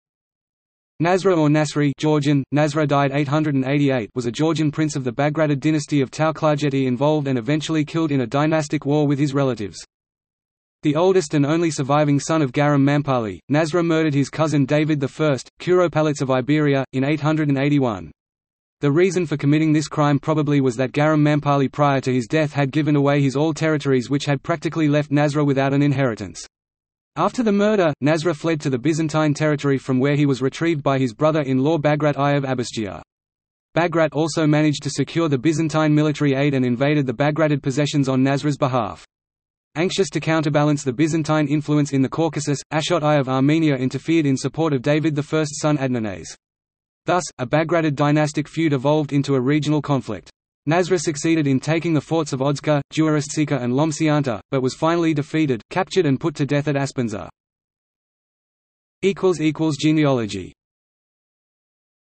Nasra of Tao-Klarjeti | Wikipedia audio article
- Socrates

SUMMARY
Nasra or Nasri (Georgian: ნასრა) (died 888) was a Georgian prince of the Bagratid dynasty of Tao-Klarjeti involved and eventually killed in a dynastic war with his relatives.
The oldest and only surviving son of Guaram Mampali, Nasra murdered his cousin David I, curopalates of Iberia, in 881. The reason for committing this crime probably was that Guaram Mampali prior to his death had given away his all territories which had practically left Nasra without an inheritance. After the murder, Nasra fled to the Byzantine territory from where he was retrieved by his brother-in-law Bagrat I of Abasgia. Bagrat also managed to secure the Byzantine military aid and invaded the Bagratid possessions on Nasra's behalf. Anxious to counterbalance the Byzantine influence in the Caucasus, Ashot I of Armenia interfered in support of David I’s son Adnarnase. Thus, a Bagratid dynastic feud evolved into a regional conflict. Nasra succeeded in taking the forts of Odzrkhe, Juaristsikhe and Lomsianta, but was finally defeated, captured and put to death at Aspindza.